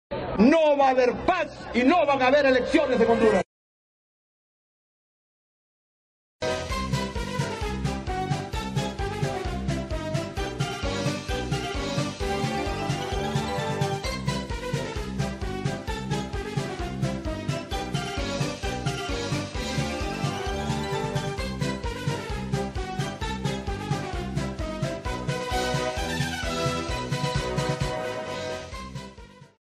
Manuel Zelaya se contradice al indicar que no iban a haber elecciones en Honduras.
Nuevamente Manuel Zelaya líder del partido libertad y Refundación (libre) contradice al indicar que no iban a haber elecciones en Honduras.